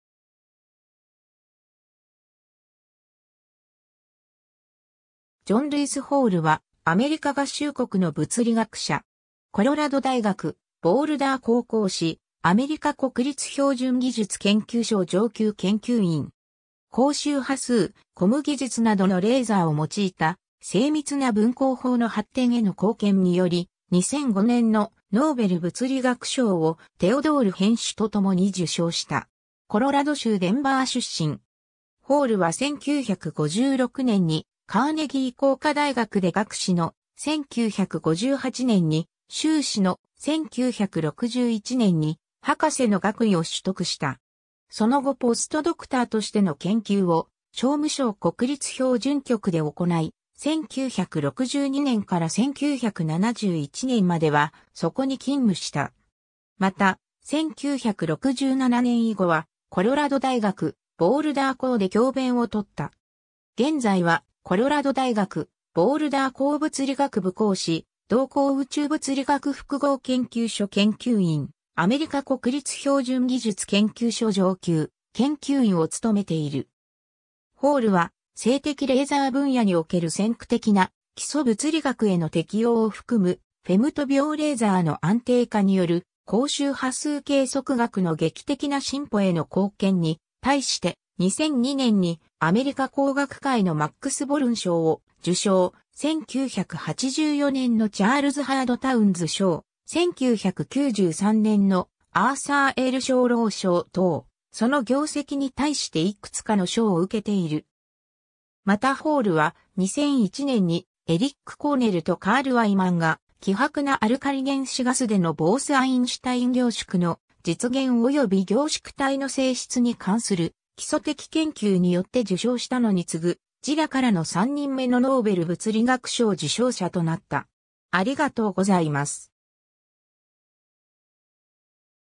ジョン・ホール (物理学者)
ジョン・ルイス・ホール（John Lewis Hall、1934年8月21日 - ）は、アメリカ合衆国の物理学者。
コロラド大学ボールダー校講師、アメリカ国立標準技術研究所 (NIST) 上級研究員。
 「光周波数コム（櫛）技術などのレーザーを用いた精密な分光法の発展への貢献」により、2005年のノーベル物理学賞をテオドール・ヘンシュとともに受賞した。
 コロラド州デンバー出身。
ホールは1956年にカーネギー工科大学で学士の、1958年に修士の、1961年に博士の学位を取得した。
その後ポストドクターとしての研究を商務省国立標準局（NBS、現在のNIST）で行い、1962年から1971年まではそこに勤務した。
また、1967年以後はコロラド大学ボールダー校で教鞭を執った。
現在はコロラド大学ボールダー校物理学部講師、同校宇宙物理学複合研究所（JILA: Joint Institute for Laboratory Astrophysics）研究員、アメリカ国立標準技術研究所上級研究員を務めている。
 ホールは、「静的レーザー分野における先駆的な、基礎物理学への適用を含む、フェムト秒レーザーの安定化による光周波数計測学の劇的な進歩への貢献」に対して、2002年にアメリカ光学会のマックス・ボルン賞を受賞、1984年のチャールズ・ハード・タウンズ賞、1993年のアーサー・L・ショーロー賞等、その業績に対していくつかの賞を受けている。
 またホールは、2001年にエリック・コーネルとカール・ワイマンが「希薄なアルカリ原子ガスでのボース＝アインシュタイン凝縮の実現および凝縮体の性質に関する基礎的研究」によって受賞したのに次ぐ、JILAからの3人目のノーベル物理学賞受賞者となった。